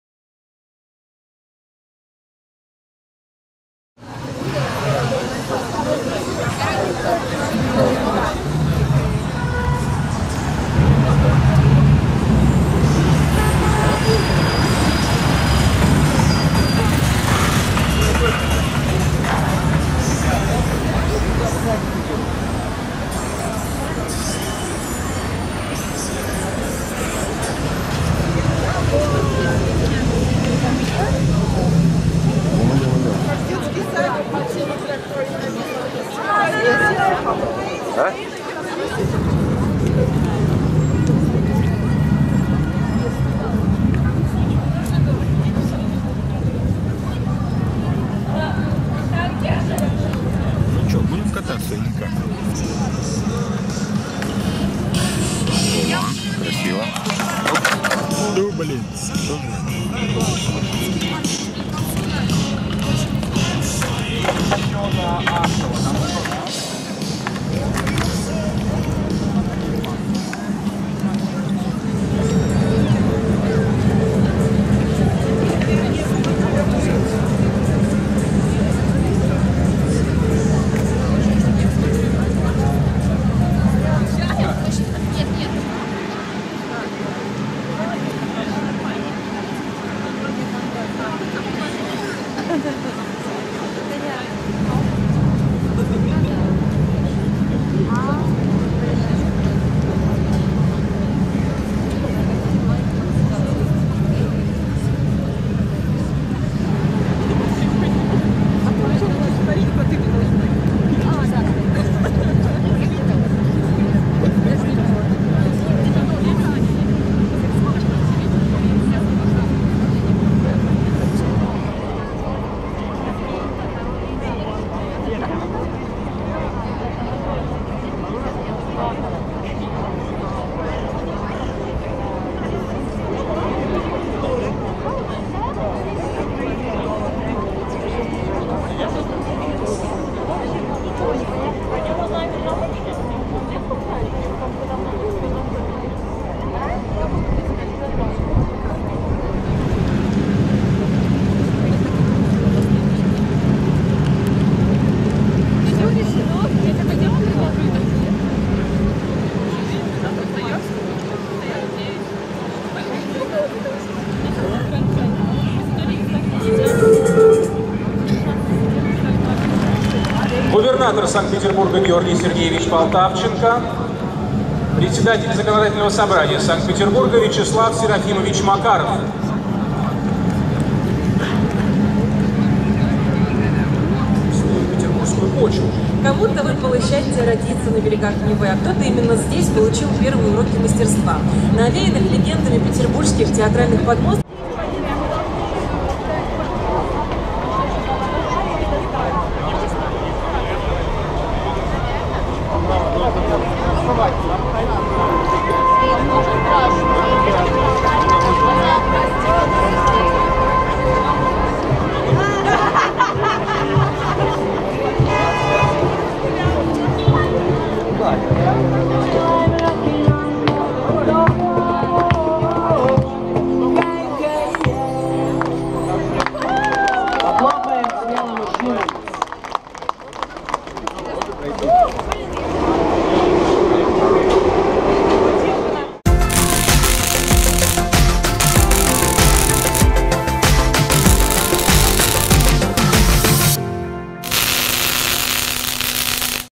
День города.Дворцовая площадь. Санкт-Петербург
Просто прогулка по площади и окресностям в День города.